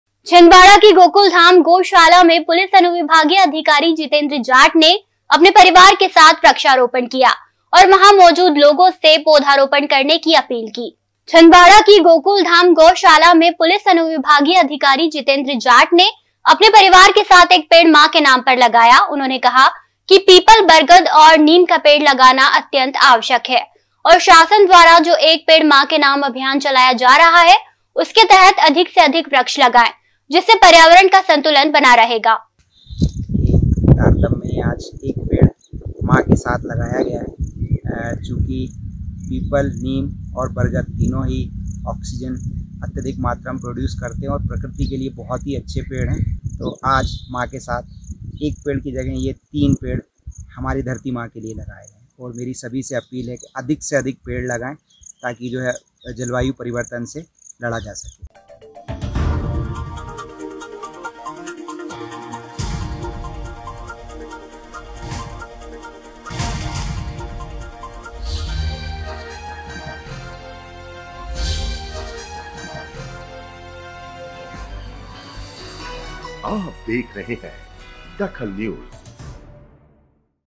DAKHAL NEWS "एक पेड़ माँ के नाम" अभियान के तहत किया वृक्षारोपण
"एक पेड़ माँ के नाम" अभियान के तहत किया वृक्षारोपण
 पर्यावरण का संतुलन बनाये रखने के लिए पेड़ आवश्यक